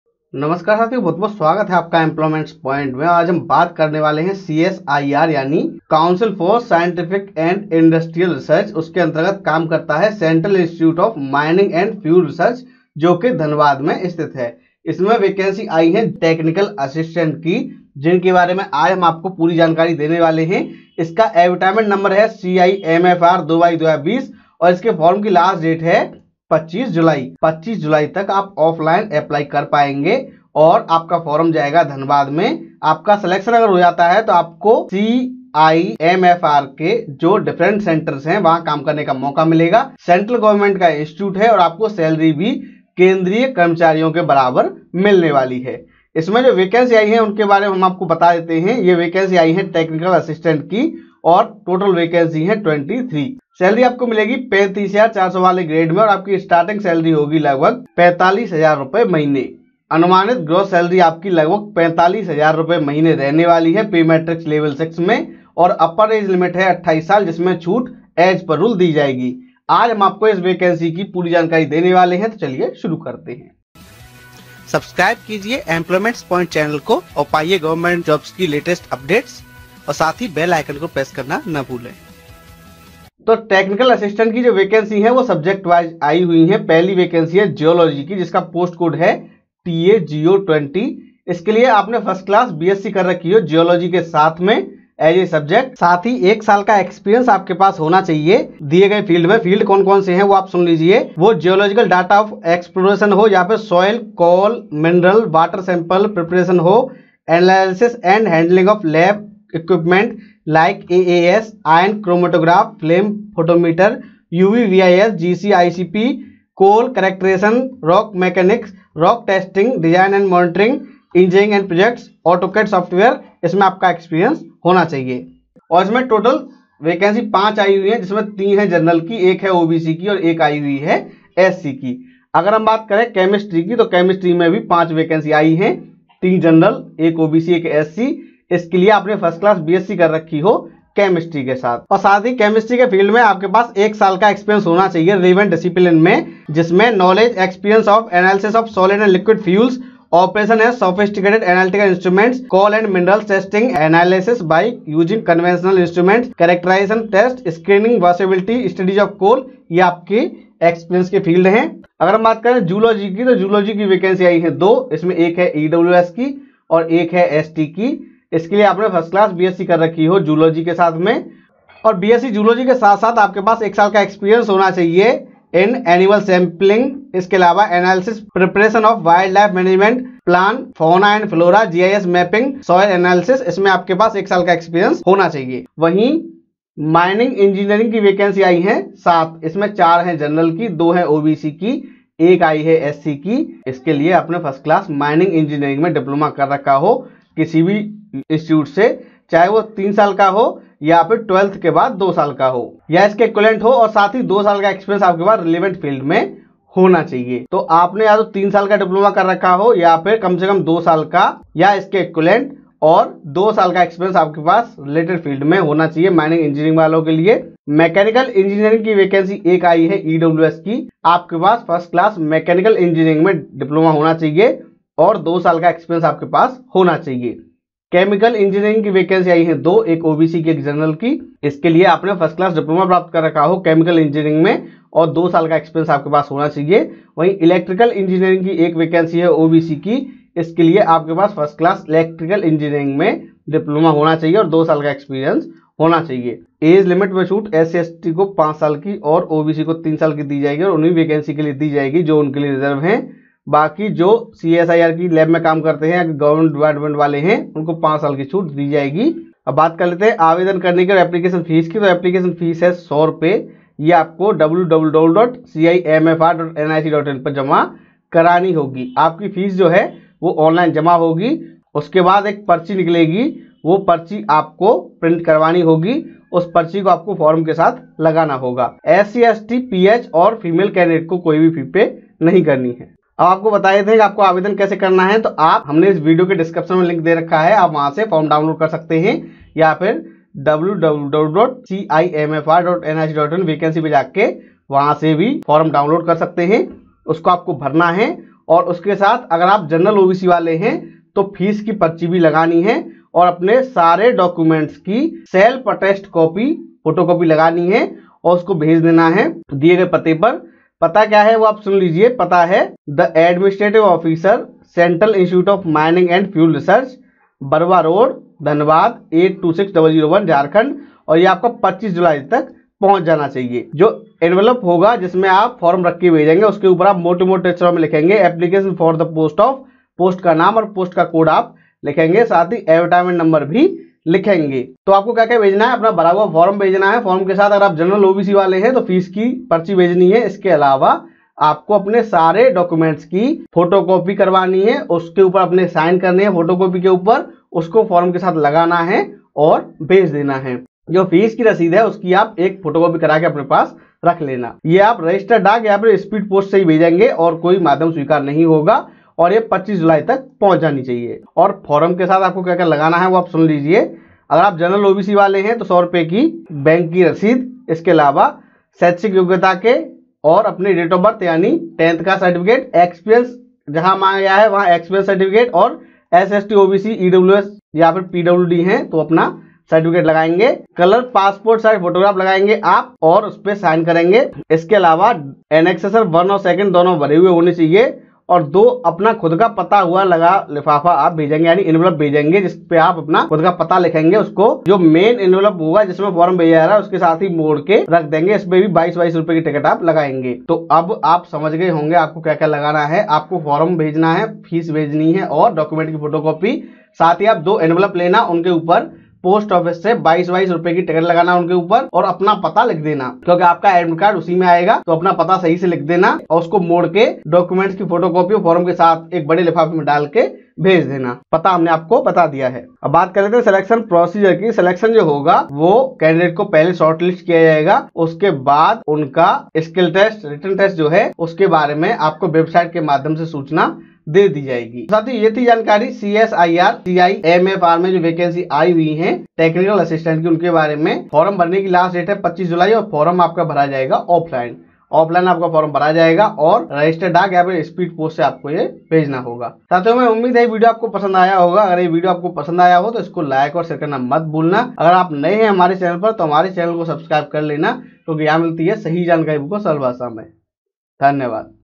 CIMFR Technical Assistant Recruitment 2020 | CSIR Technical Assistant Recruitment 2020
Name of the post : Technical Assistant

CIMFR Technical Assistant Recruitment 2020 Age limit : 28 years as on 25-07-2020. Upper age relaxation as per rule.

CIMFR Technical Assistant Recruitment 2020 Qualification : Degree/ Diploma  in relevant field and experience.

CIMFR Technical Assistant Recruitment 2020 salary : 35400/- plus allowances.

CIMFR Technical Assistant Recruitment 2020 subject wise vacancy :

CIMFR Technical Assistant B.Sc. Vacancy :
Geology - 5
Chemistry -5
Zoology - 2

CIMFR Technical Assistant Diploma Vacancy

Mining Engineering - 7
Mechanical Engineering - 1
Chemical Engineering - 2
Electrical Engineering - 1

How to apply : Application form is offline. and download the Application Form. Application in the prescribed format may be submitted on A4 size thick/Xerox paper.

Application fee : Rs.100/- for UR/OBC/EWS

How to pay fee : Candidates are required to pay an Application Fee (Non-refundable) of Rs.100/-only (Rupees One Hundred only). Payment must be made through SBI payment gateway only available at CIMFR After making necessary payment, the candidate must enclose the web generated slip along with the application form.